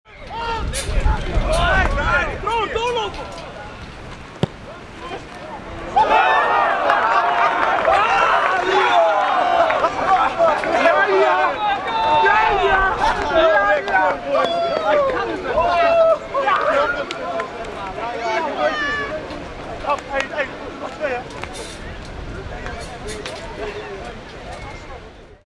FIFPro Tournament 2011: Holland-Portugal 3-0
FIFPro Tournament 2011: Hans Denissen scoring the third Dutch goal by a beautiful free kick à la Cristiano Ronaldo! Holland - Portugal 4-0